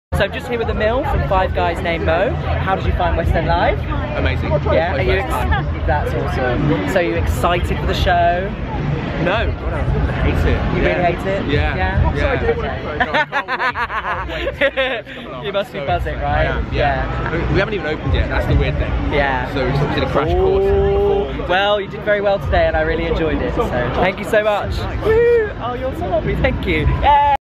West End Live 2017 - Perry O'Bree with... Emile Ruddock
StageFaves vlogger Perry O'Bree with Emile Ruddock, opening soon in Clarke Peters' West End revival of Five Guys Named Moe at the specially built Marble Arch Theatre, at West End Live on 24 June 2017.